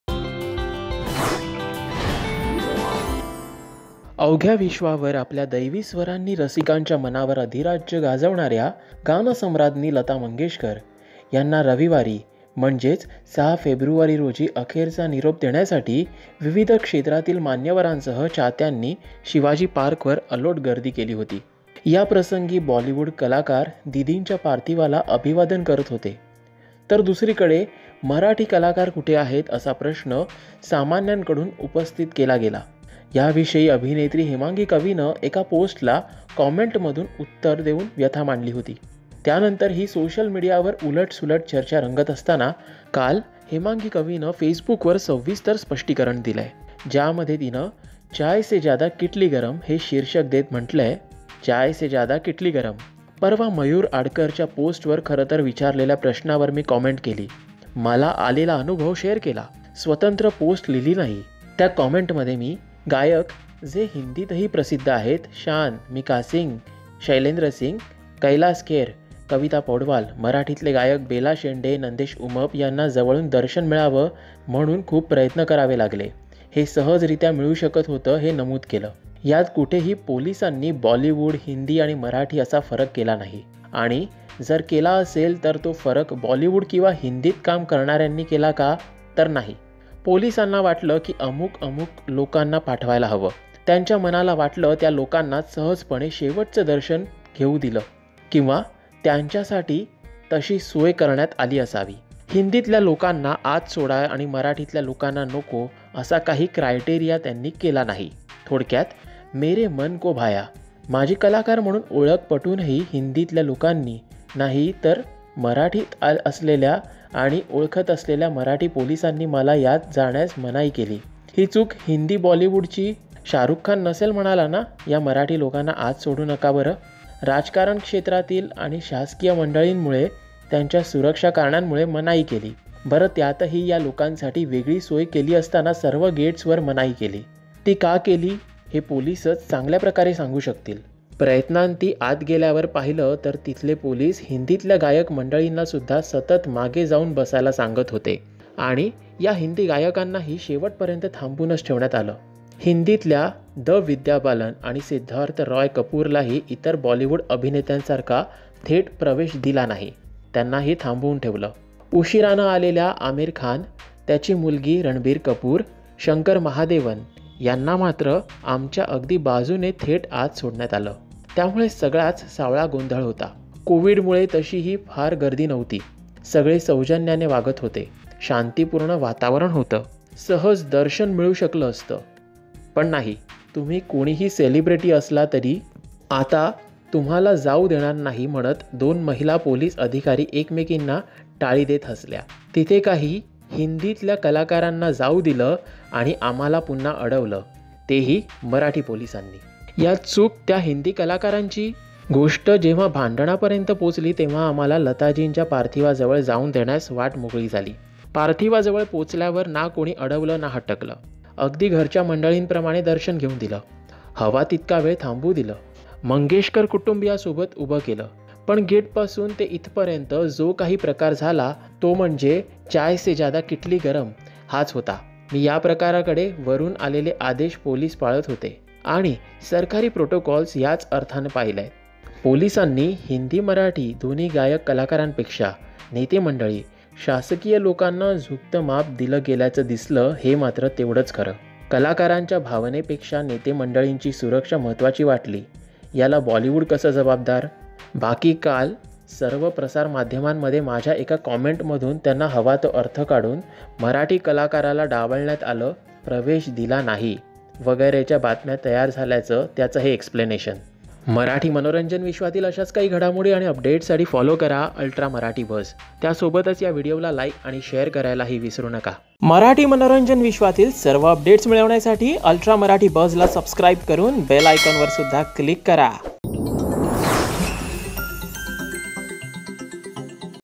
त्यावेळी जे घडलं त्याला बॉलिवूड कसं जबाबदार?| Hemangi Kavi Interview| Bharatratna Lata Mangeshkar
त्यावेळी जे घडलं त्याला बॉलिवूड कसं जबाबदार ? | Hemangi Kavi Interview |  Bharatratna | Nightangle Of India | Lata Mangeshkar No More

अवघ्या विश्वावर आपल्या दैवी स्वरांनी रसिकांच्या मनावर अधिराज्य गाजवणाऱ्या गानसम्राज्ञी , भारतरत्न लता मंगेशकर काल अनंतात  विलीन झाल्या. त्यांच्या अंत्यदर्शनासाठी अलोट जनसमुदाय शिवजीपार्क वर लोटला होता. सोशल मीडियावर उलट सुलट चर्चा रंगत असताना काल  हेमांगी कवीनं फेसबुक वर सविस्तर स्पष्टीकरण दिली आहे. याविषयी सविस्तर जाणून घ्या "अल्ट्रा मराठी बज"वर ! Report & Voice Over By Sagar Maladkar Video Editor - Ajay Shukla